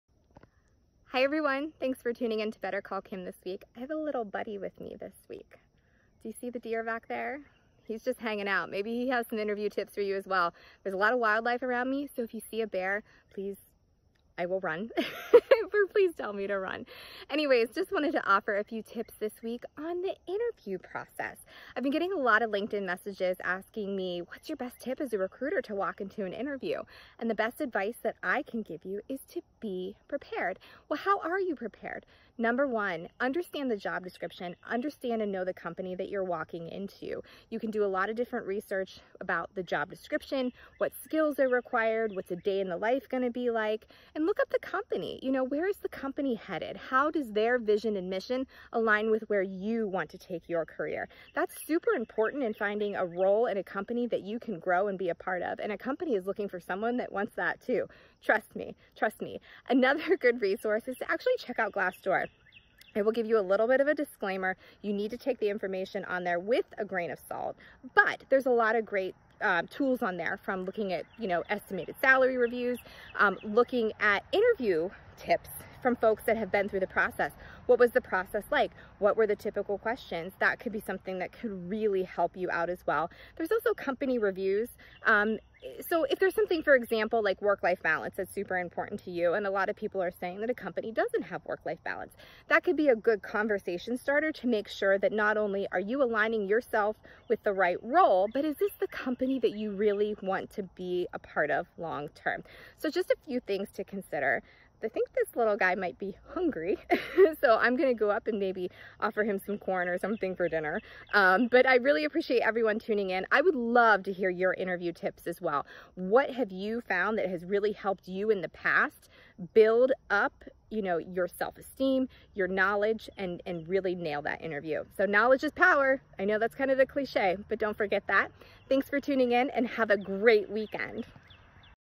Oh deer! How can you nail your next interview?
Tune in to bettercallkim this week for some quick and easy tips to nail your next interview!

What tips do you have to share? Let’s get the conversation started, and harness the power of LinkedIn!